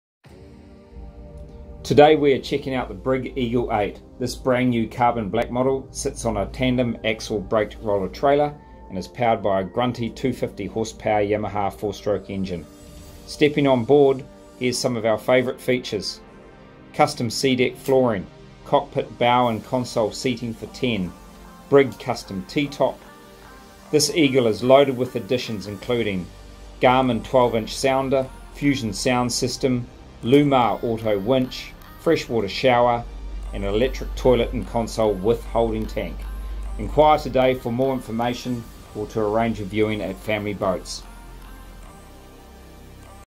Brig Eagle 8
Take a virtual tour of our Brig Eagle 8.

The Eagle 8 is BRIGs luxury family cruiser with a sleek, iconic profile and stylish design that stands out in any situation. An extended bow can be equipped with either a generous sundeck or an additional seating area, helping make the Eagle 8 the ultimate family RIB on the market.